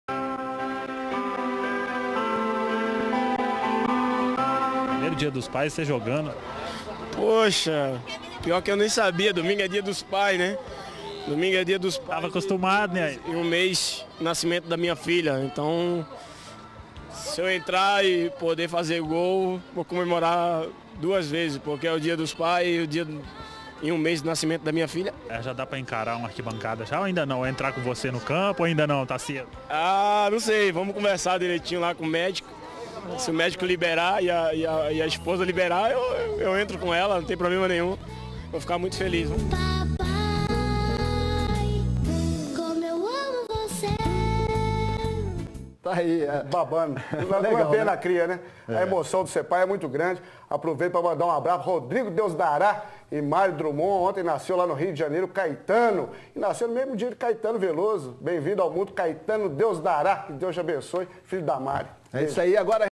Luan quer fazer gol no Dia dos Pais
Luan quer fazer gol no Dia dos Pais. Sobretudo, porque há um mês ele vive a experiência de também ser pai. Na Zona Mista, o craque do Atlético disse que se concretizar o gols, ele comemorará duas vezes. Veja: